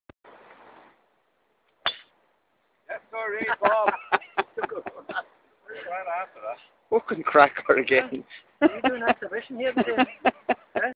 BD President does 'Happy Gilmore'1
BD Golf Society President, Mr Sean Walsh, 74 years 'young', doing 1 of his many Happy Gilmores! Excuse my language, but it was a cracker, straight down the middle!